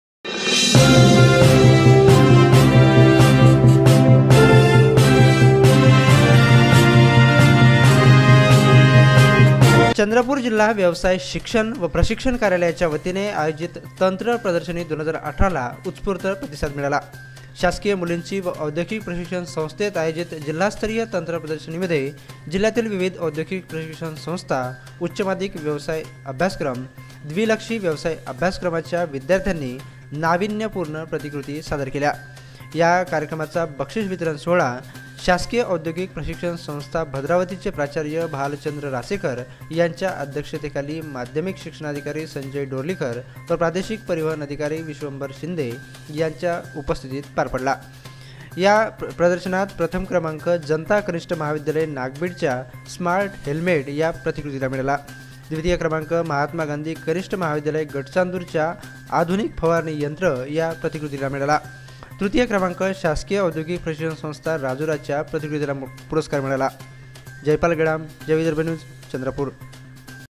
चंद्रपुरातील तंत्र प्रदर्शनाला उत्स्फूर्त प्रतिसाद
नागभिडची जनता कनिष्ठ महाविद्यालय प्रथम